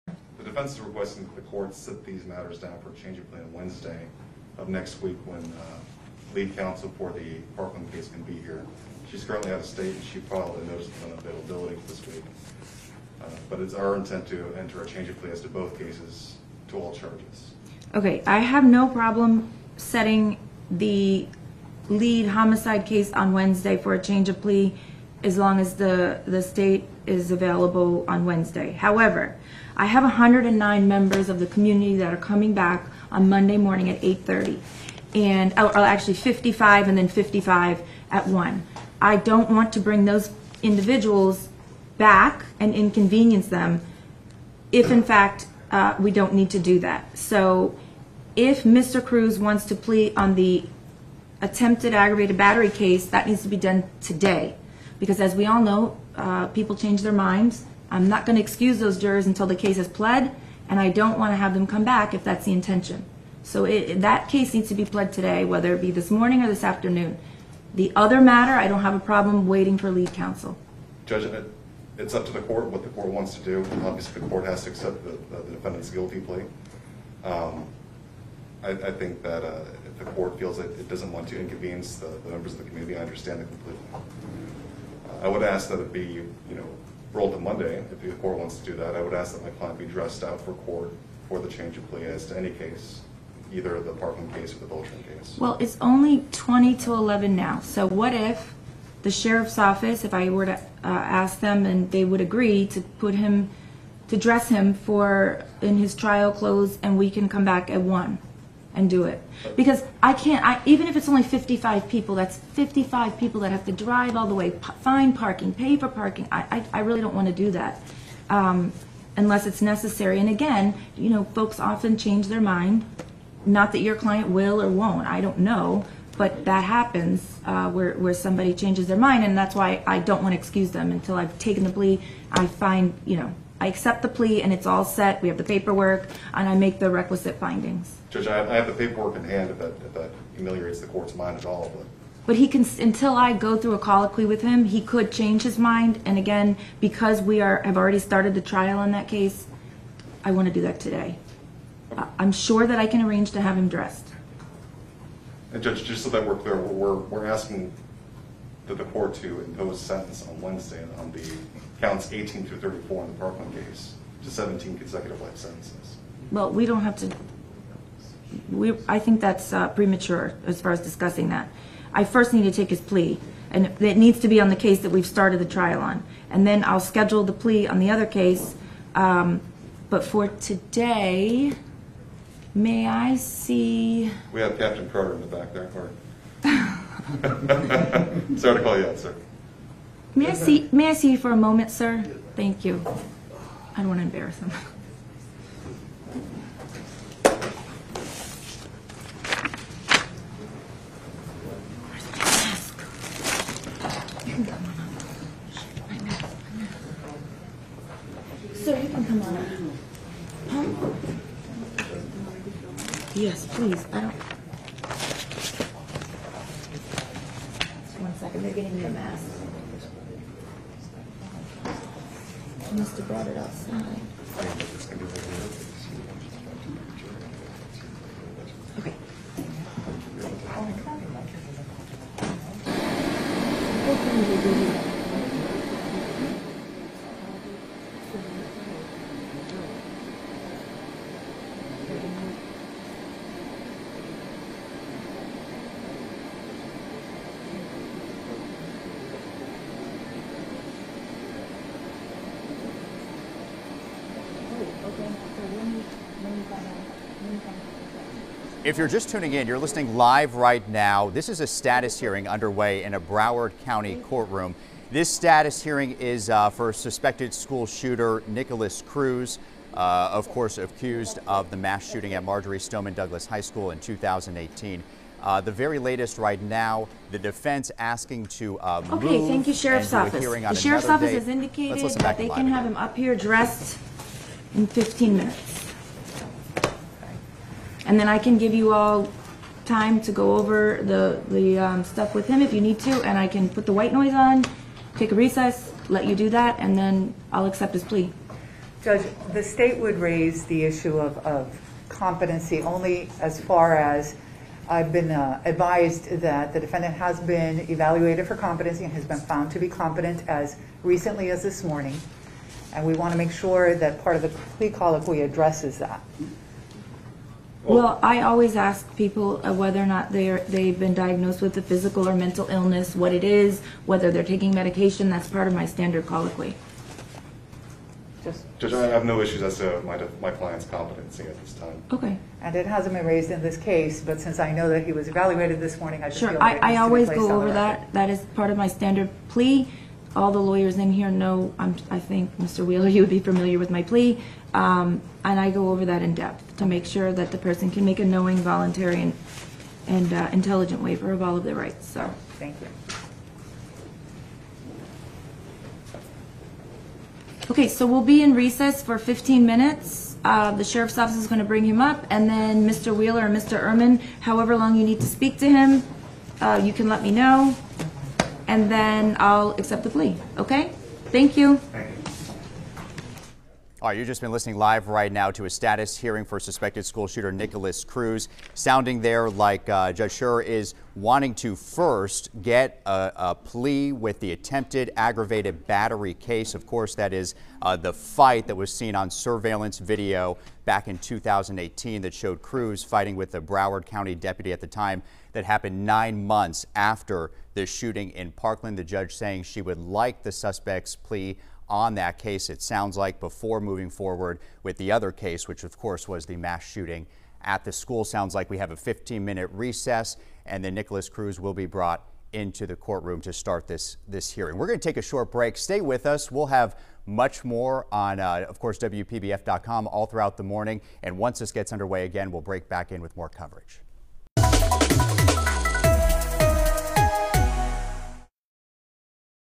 Status hearing in Nikolas Cruz trial
A status hearing was held Friday morning in the Nikolas Cruz trial.